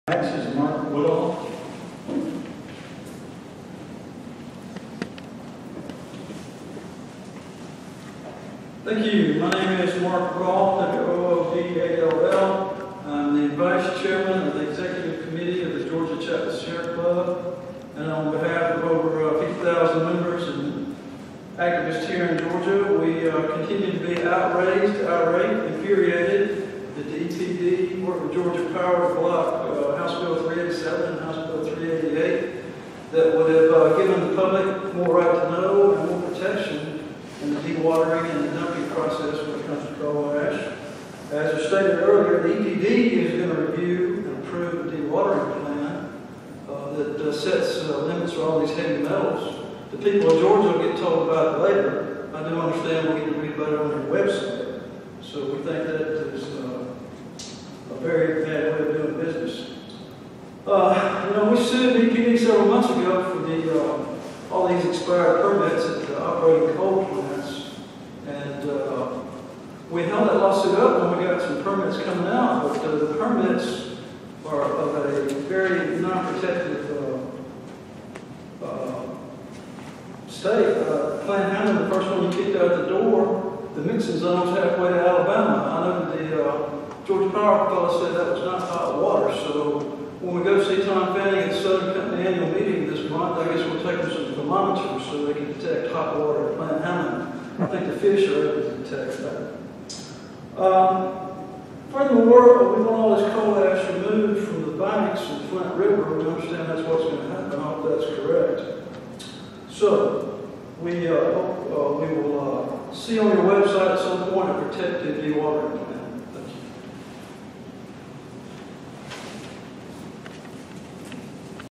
Mark Woodall @ EPD Permit Hearing - 2017-05-04
Georgia EPD Public Hearing on Plant Mitchell Coal Ash Pond Permitting - Albany Georgia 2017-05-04